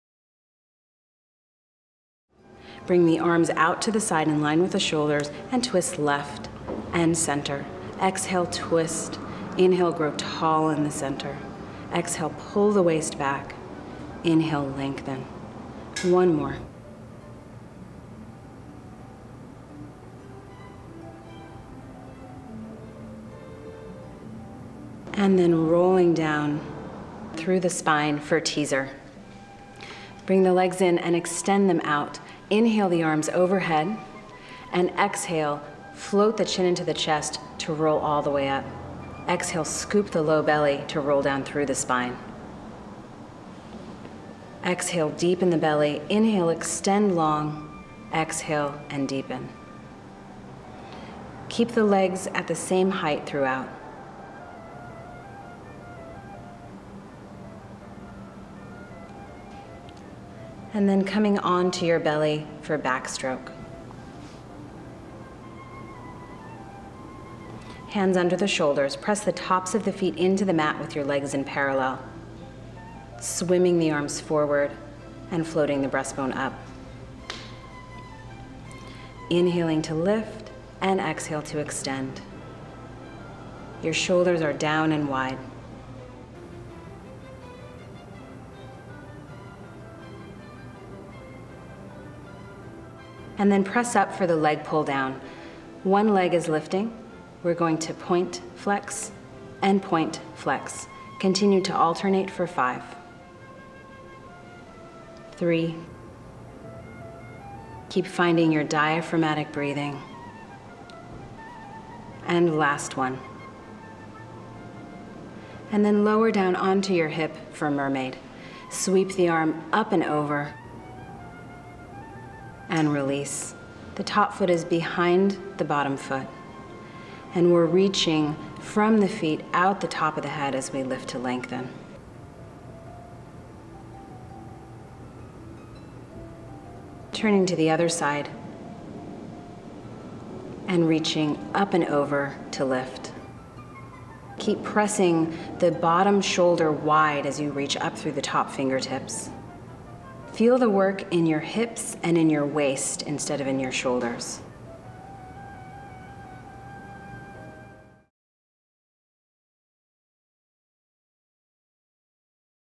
Erika Bloom demonstrates the Pilates advanced flow exercises
"Next-Level Pilates with Erika Bloom" offers targeted routines for the core, upper body, and lower body that can be completed with simple at-home equipment. The DVD includes nine 10-minute routines and a programming matrix that can be used to create personalized workouts based on individual fitness goals, experience levels, or time constraints.